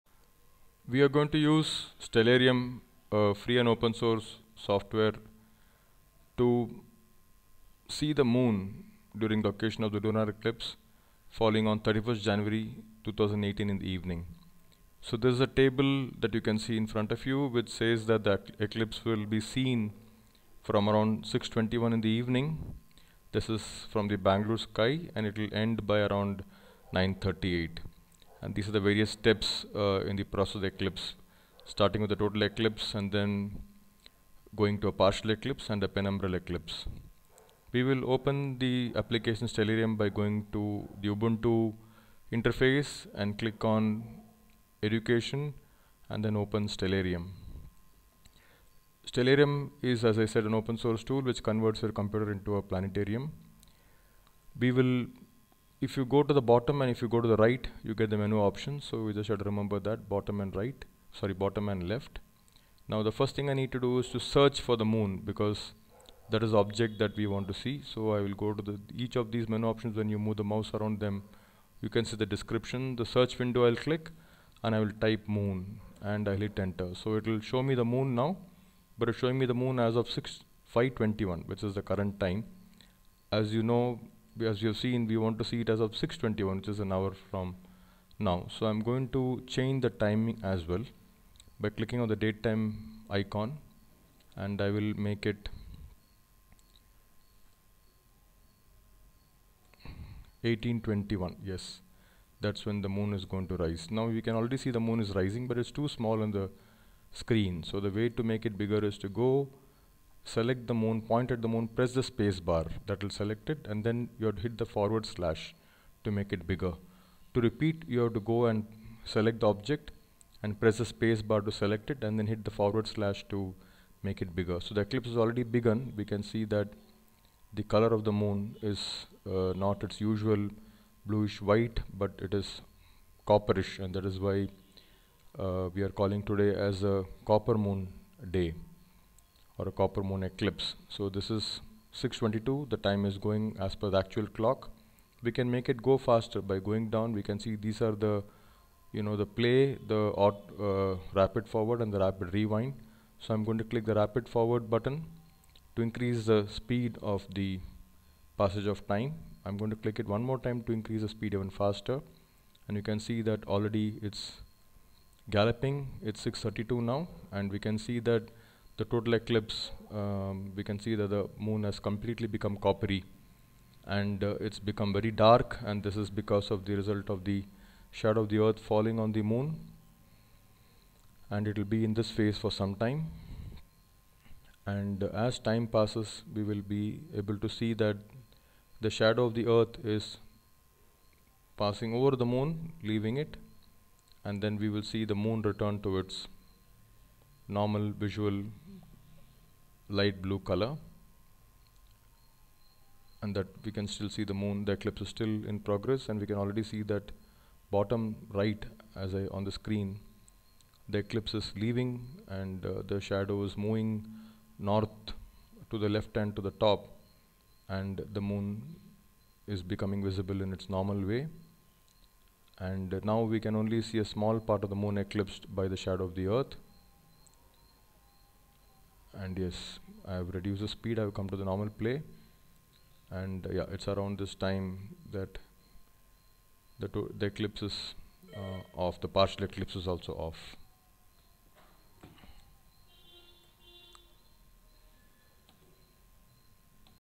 Total Lunar eclipse Jan 31, 2018 IT for Change, Bengaluru recording of Stellaruim and Kazam CCBYSA
This video shows the Total Lunar Eclipse seen on the Stellarium free and open source software, for Bengaluru location on January 31st 2018.
We have  recorded this video using Kazam FOSS screen casting software on Ubuntu 16.04 Operation System.